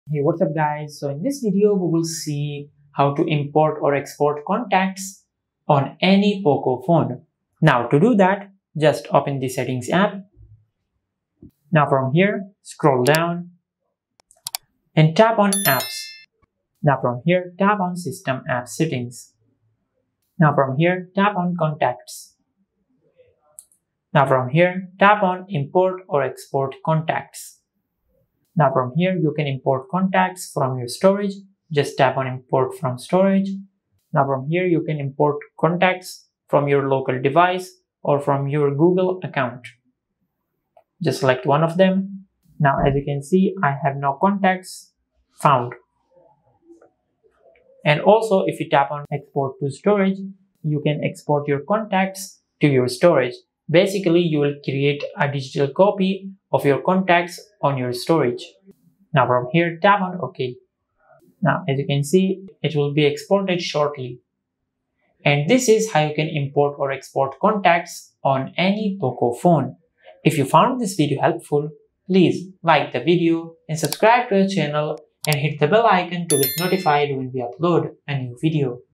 How to Import/Export Contacts on Any Poco Phone
How to import/export contacts on any poco phone. To change contacts settings on any poco phone, follow these steps.